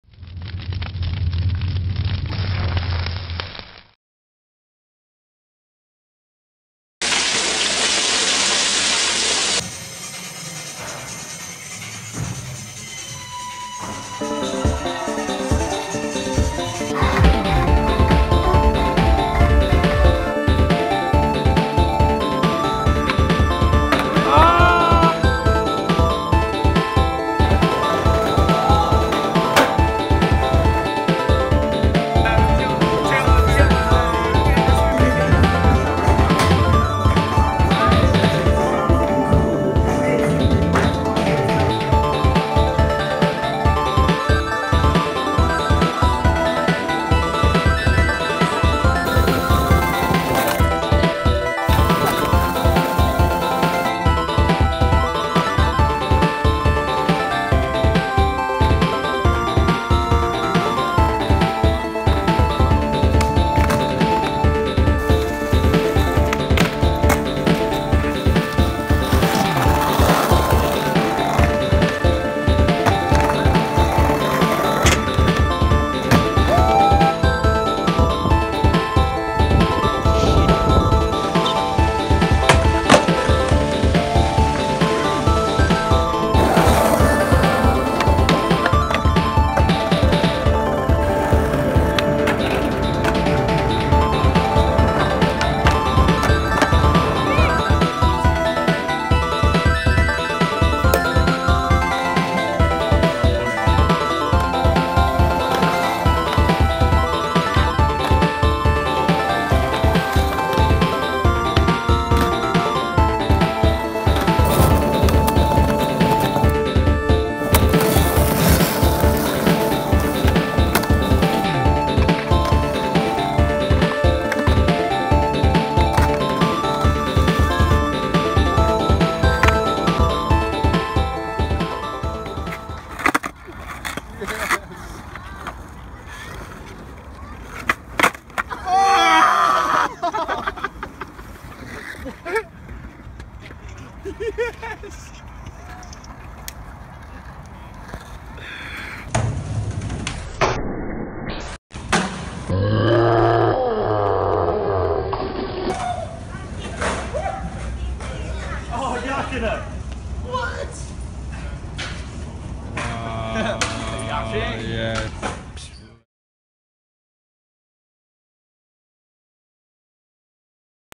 Mind of Marius: Asia
The mind and body of Marius are always on the move, this time touching down in the Far East. From epic spots to underground subways, the good times keep rolling and rolling.